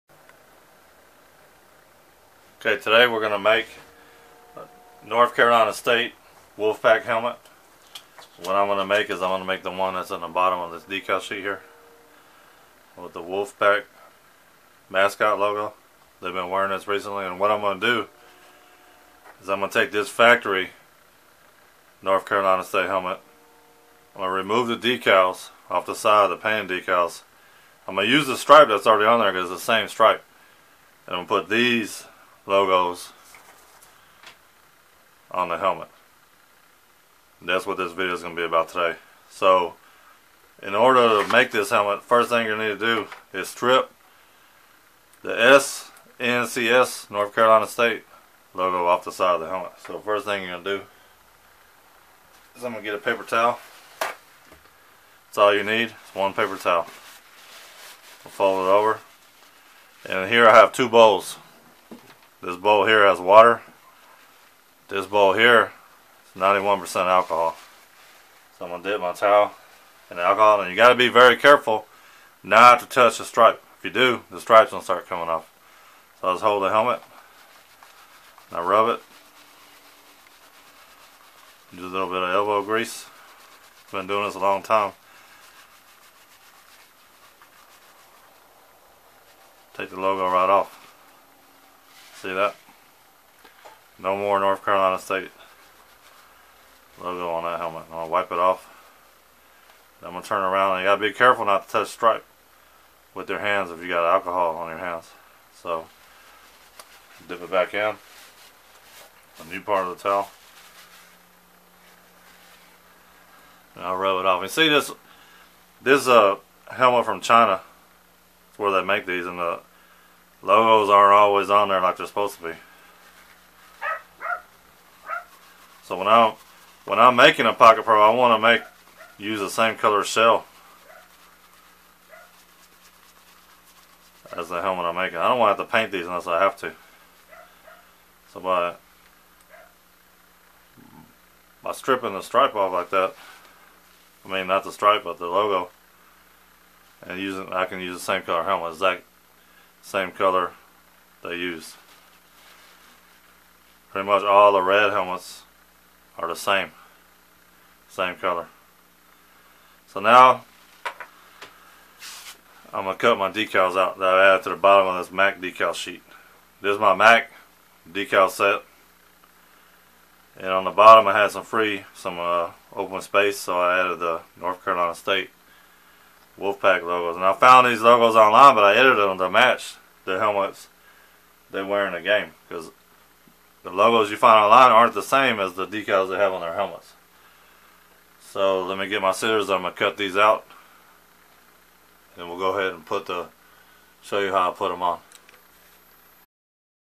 Making A Custom Pocket Pro Helmet Part 1 NC State Wolfpack
Quick 2 part video that shows me making the NC State Wolfpack 2017 mascot logo pocket pro helmet.